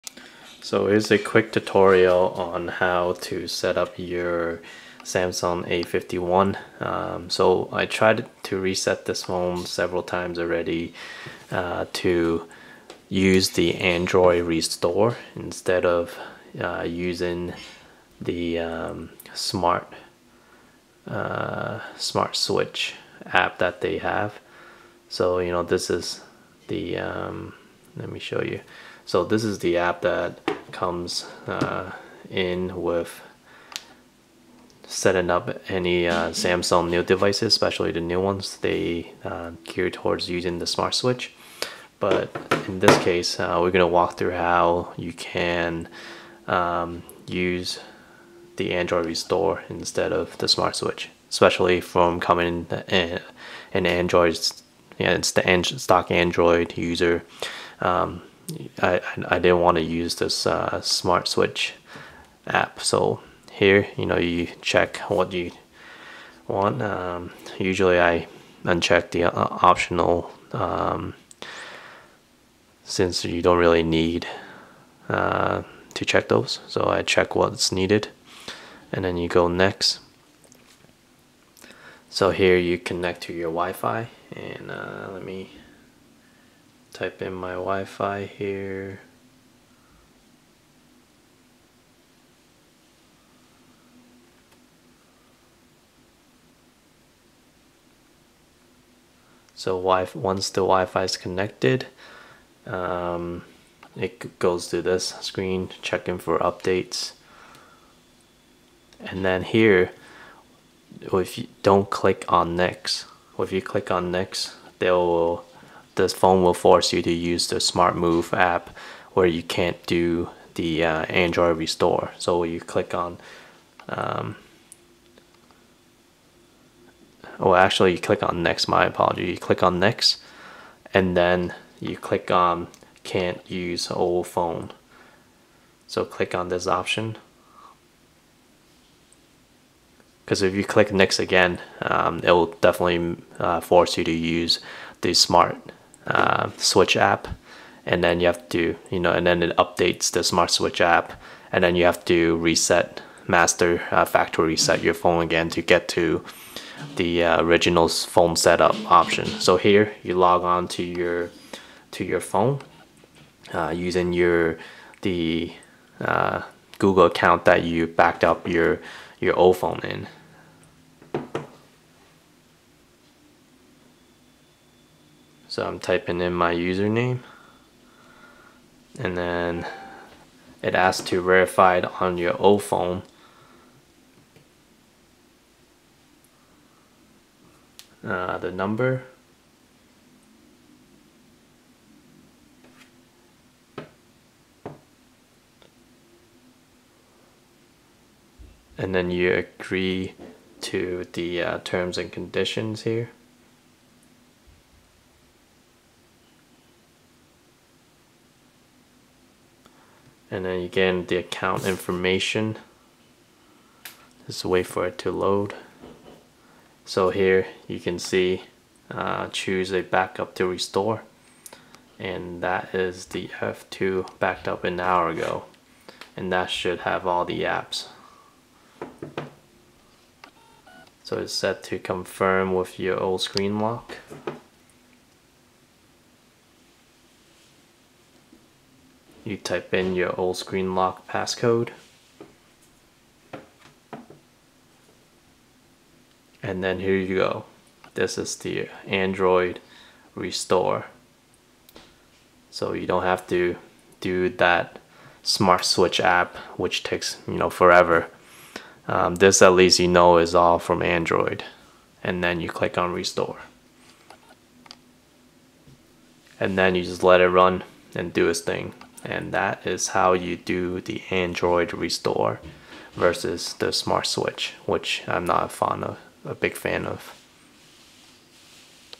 Setting up your Samsung A51 with Android Restore instead of Smart Switch App from Samsung.
Here is a video to show you how to do the Android Restore option (Choose a backup to restore) on the Samsung A51...I did perform a back up on my old device, but I didn't see the page where I can do the google Android restore of all my apps, contacts, calendar, messages, etc.  All I saw was the Smart Switch Option on the Samsung Start up.

I factory reset my phone like 10 times and finally figured it out. Hope this helps all of you. Hope this will help everyone who's trying to switch to Samsungs but doesn't want to use the Smart Switch App!

Hope everyone's liking their samsung A51s!

Cheers!